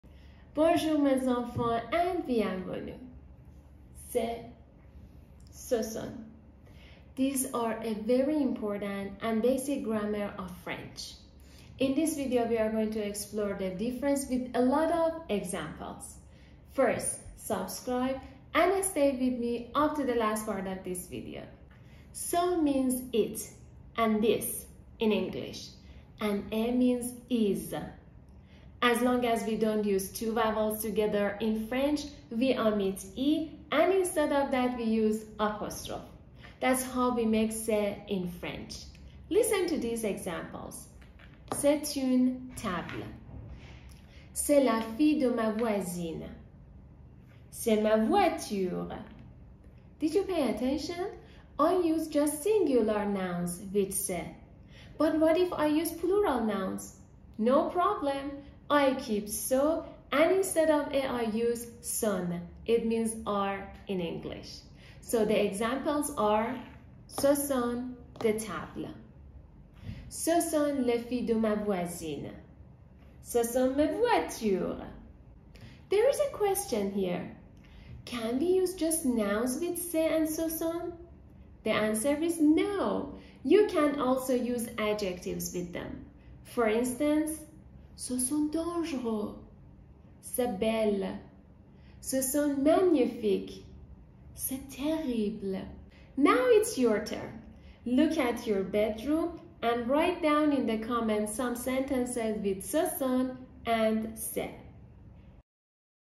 C'est vs. Ce Sont: Understanding the Key Difference in French
Unlock the secrets of French grammar with our in-depth tutorial on the distinction between "c'est" and "ce sont." 🇫🇷 Whether you're a beginner or looking to refine your skills, this video provides a clear and concise explanation. Enhance your understanding of the French language and elevate your communication. Dive into the world of French grammar now!

Here's what you can expect:
00:00-00:22 intoriduction
00:22-00:56 what is "c'est"
00:56-01:15 What is "ce sont"
01:15-01:36 How to use  "c'est" and "ce sont"
01:36-01:47 homework